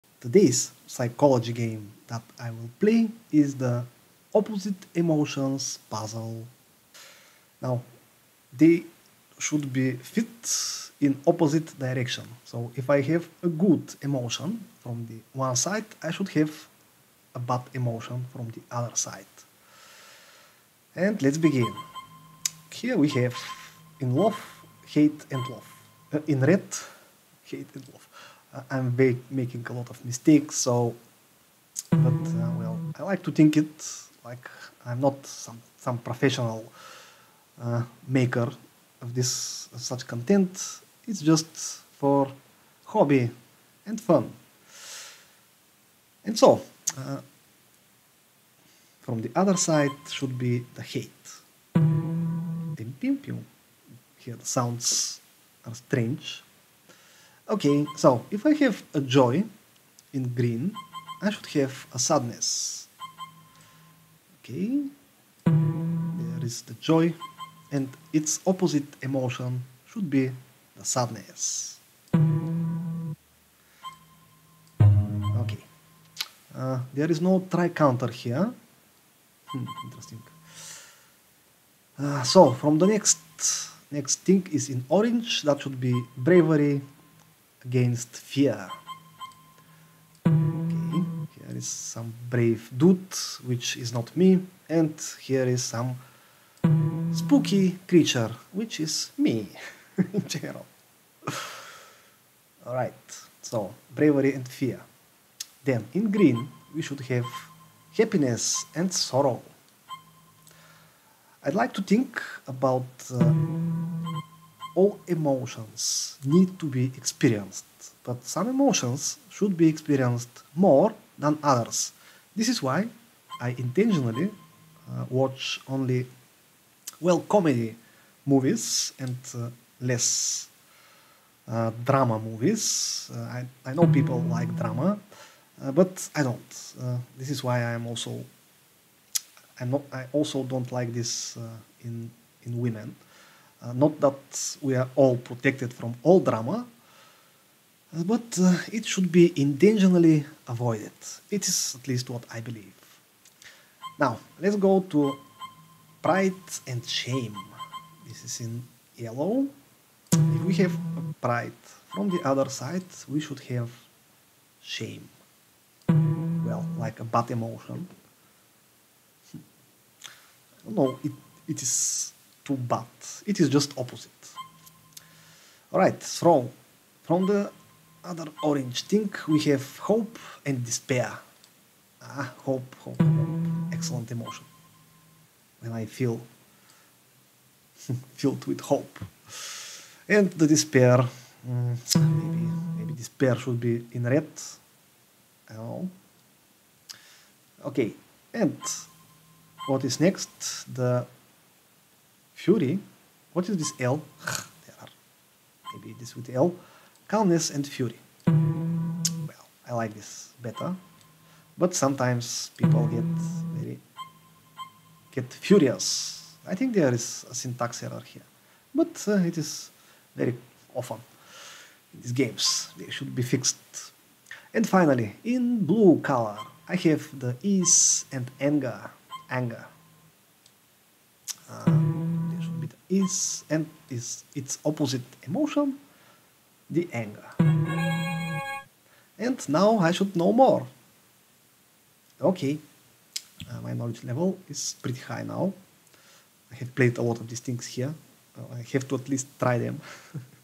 Opposite Emotions - Interactive Psychology Lesson 🧠🎥🎓
A short psychology class by master Ogiwon :)
Today's topic - Exploring Emotions.